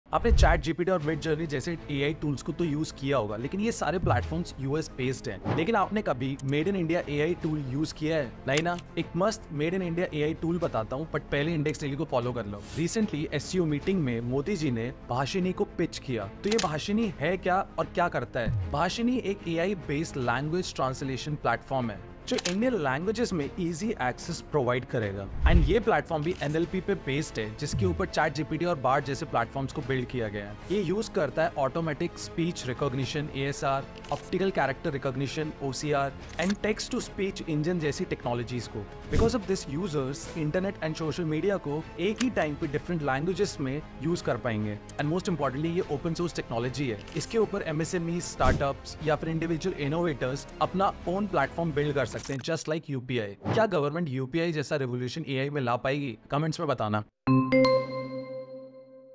Try this Made in India AI
You might have heard of AI tools like ChatGPT, Bard and Midjourney. And probably you have used them as well. But have you heard of Made in India AI tool Bhasini?

Bhashini is an Indian AI tool that aims to make the internet accessible in more Indian languages. Bhashini provides real-time translation from English to 20+ Indian languages. It is already integrated into UPI123 to make it more accessible.

Have you used Bhashini AI yet?